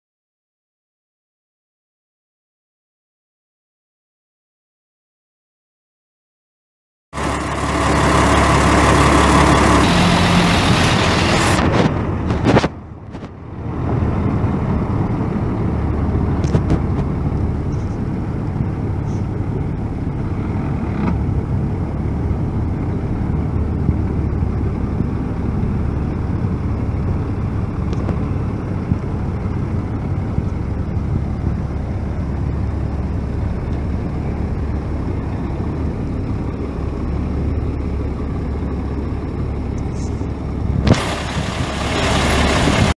2001 Sterling L9500 semi truck for sale | no-reserve Internet auction March 15, 2018
View a detailed listing of this item and place bids

2001 Sterling L9500 semi truck
797,105 miles on odometer
Single axle
AC and heat
Cruise
AM/FM/cassette
Block heater
Exterior visor
Mud flaps
Air suspension
Air brakes
11R22.5 tires
Oklahoma title
2FWKEWEBX1AH44592
It's located in Barnsdall, OK, and we're selling it to the highest bidder regardless of price.